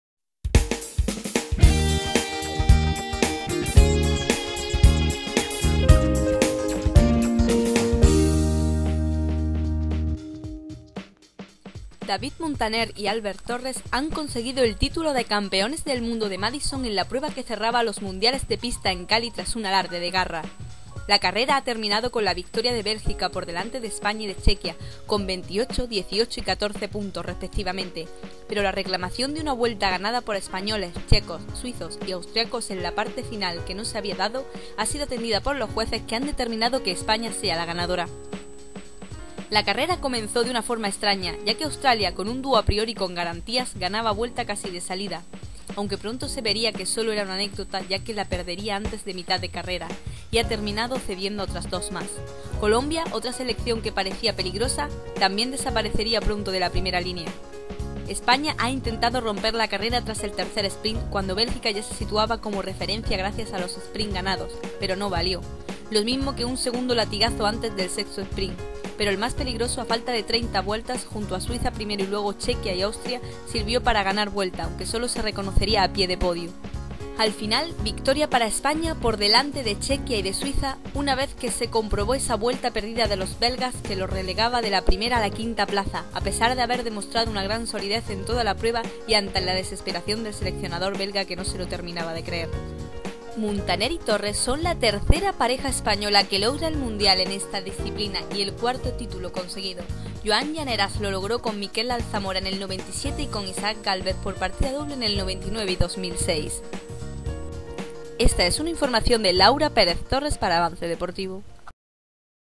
Muntaner y Torres, oro en Cali
David Muntaner y Albert Torres han conseguido el título de campeones del mundo de madison en la prueba que ha cerrado los mundiales de ciclismo en pista en Cali. Autora: Laura Pérez Torres/Imágenes: UCI